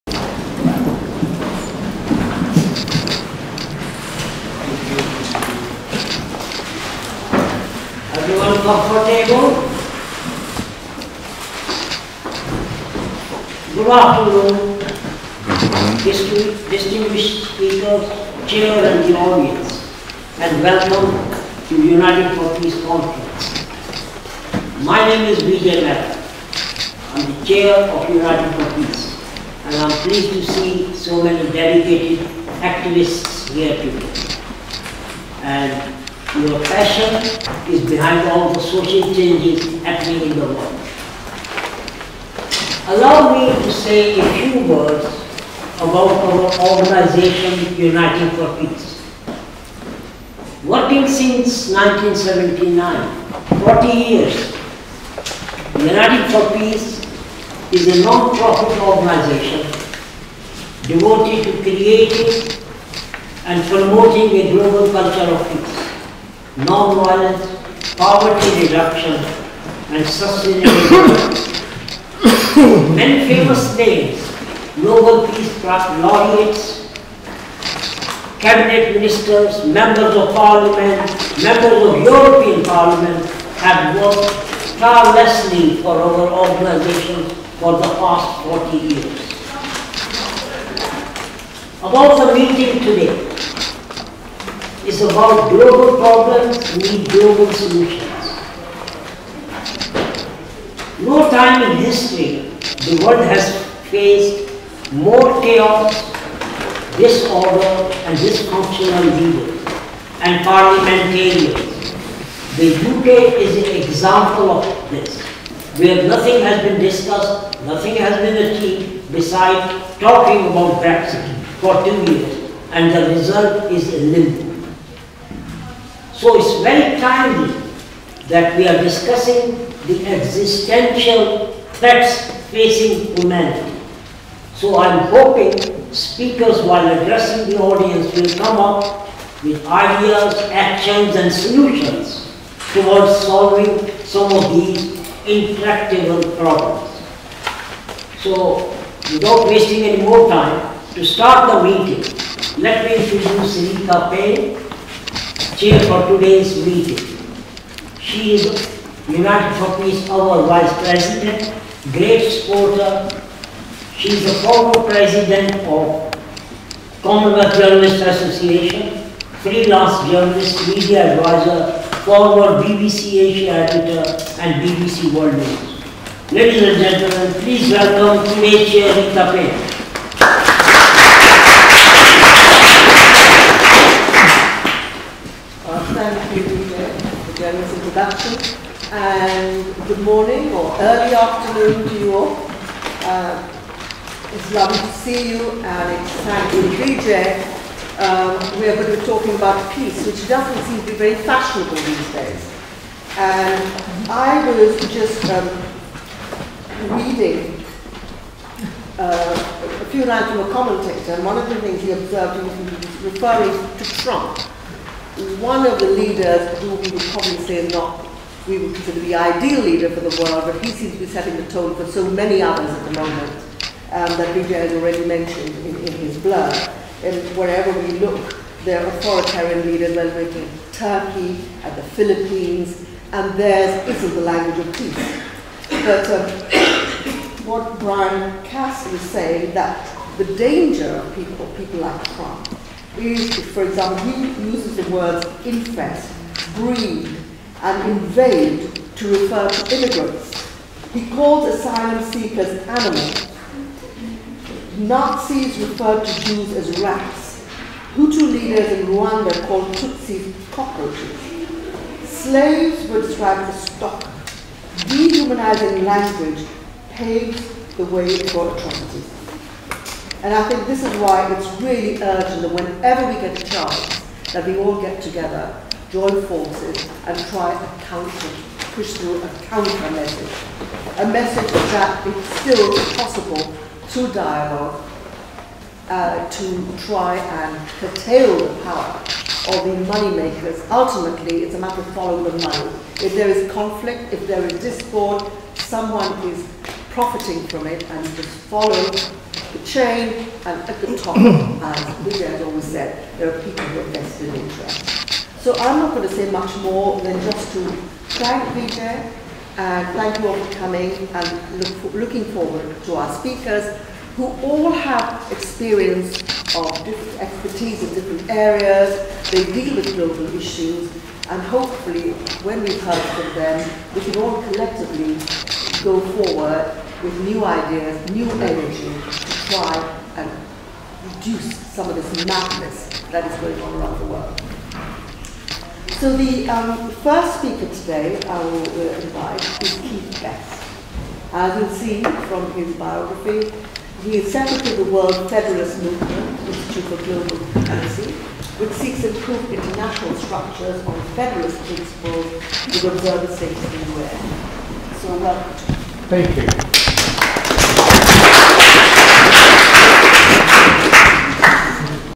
Uniting for Peace Spring Conference, 6 April 2019 - Part 1
Part 1 - Uniting for Peace Spring Conference, 6 April 2019 held at Wesley's Chapel, 49 City Road, London EC1Y 1AU.
Chair - Rita Payne
Speakers included:
Keith Best, Vijay Mehta, John Elliot, Ahmad Shahidov, Rev Brian Cooper, Caroline Millman, Sally Milne, Sanjay Mehta and Bernie Holland.

Video by Bernie Holland